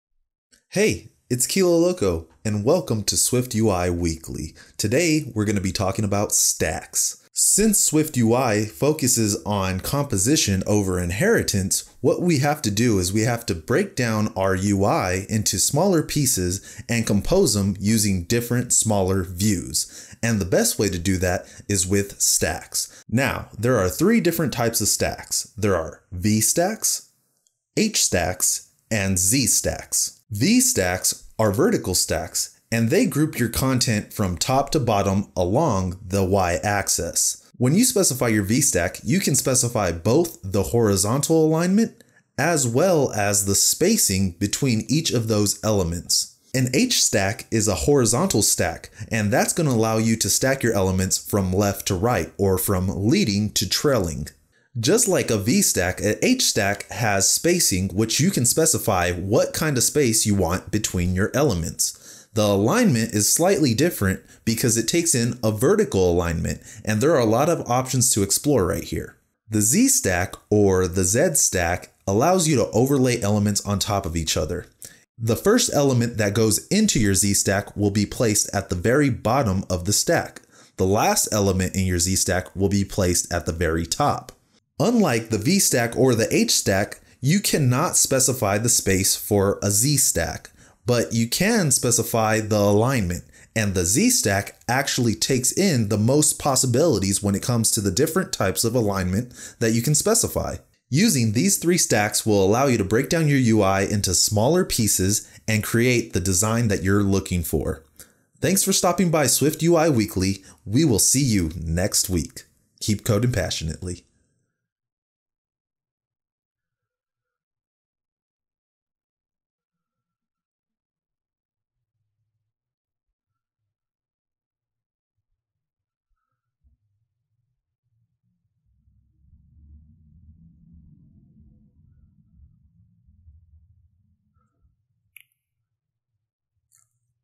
Stacks | SwiftUI Weekly
Today in SwiftUI Weekly, we are covering the different types of stacks that are provided by SwiftUI. We will be going over VStacks, HStacks, and ZStacks; and how they can be used to compose different elements to perfect your UI.

GEAR

Code Passionately T-Shirt

Laptop - 2017 MacBook Pro

Microphone - PowerDeWise Lavalier Microphone